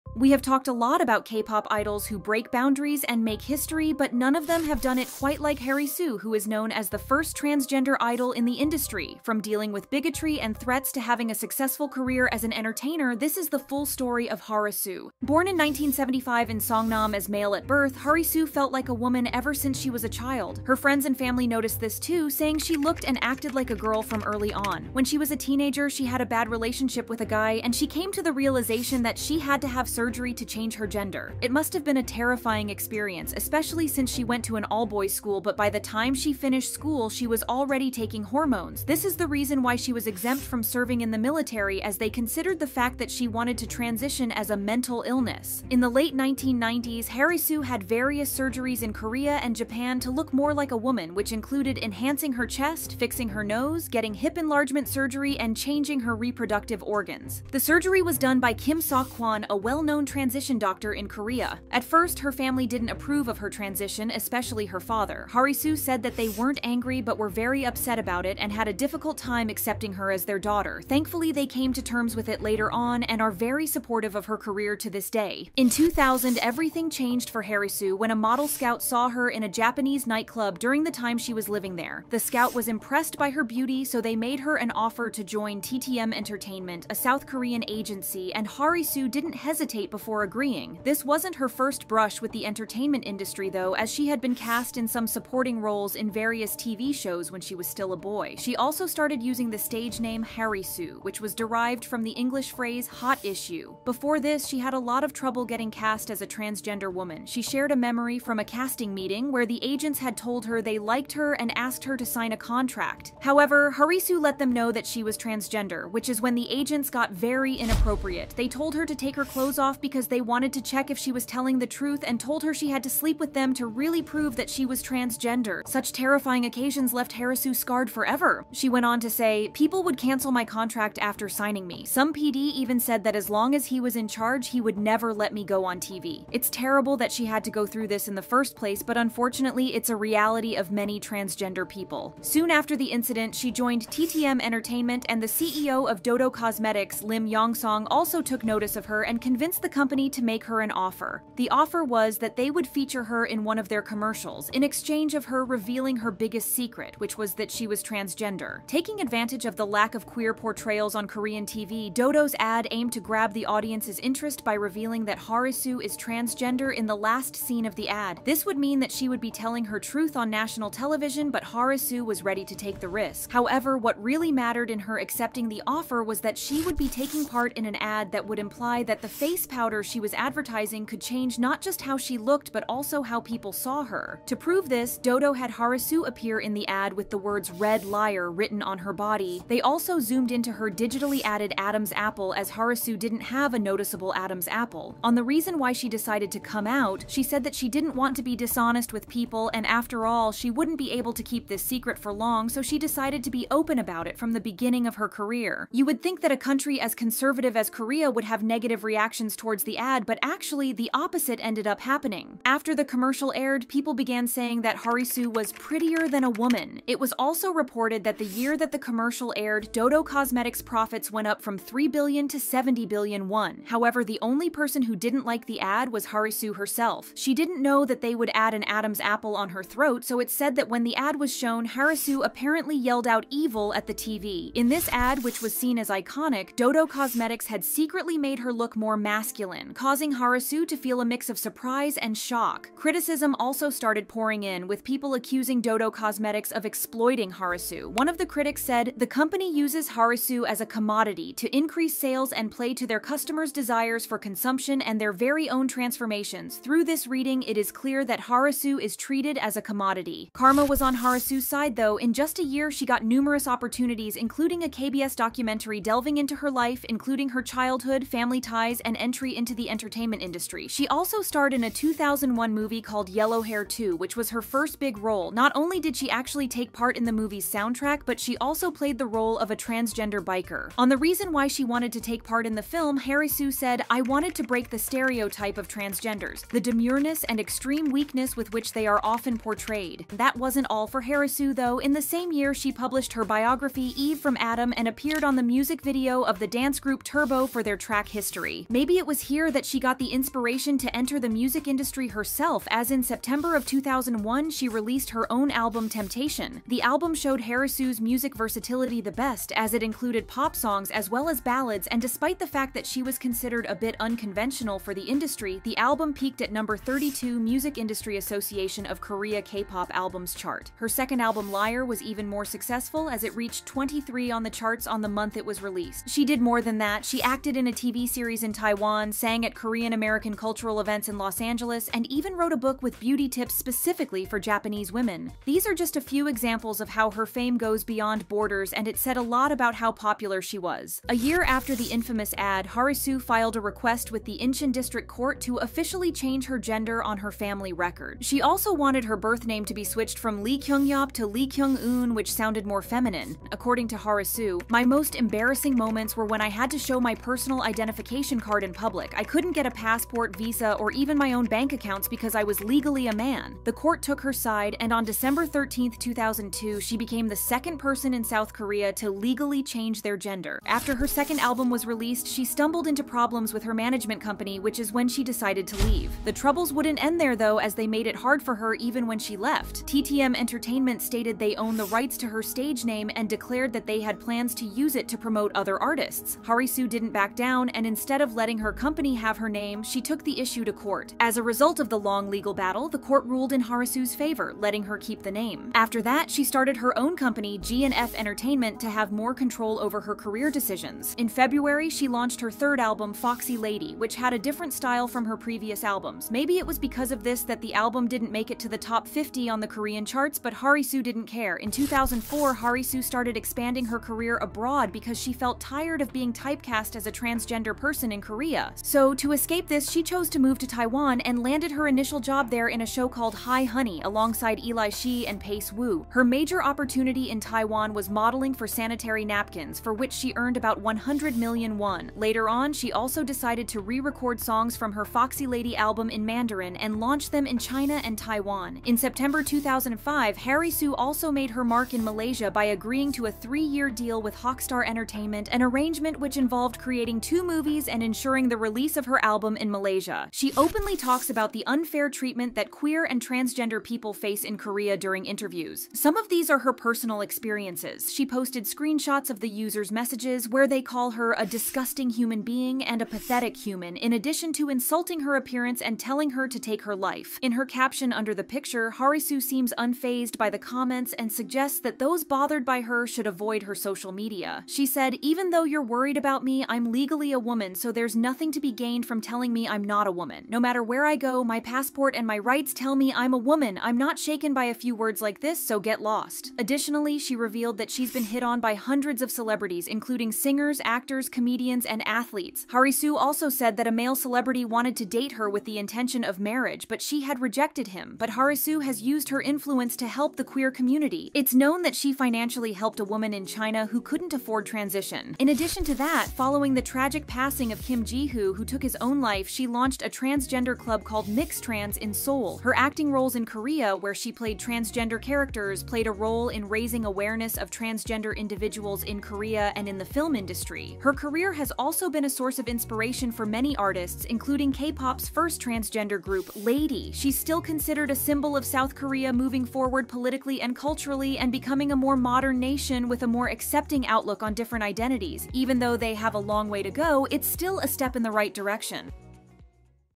The Tragic Story of the First Transgender K-Pop Idol
Harisu wrote history in Korea as she became the second person in the country to legally change their gender and the first ever transexual K-Pop idol. But her journey was far from easy.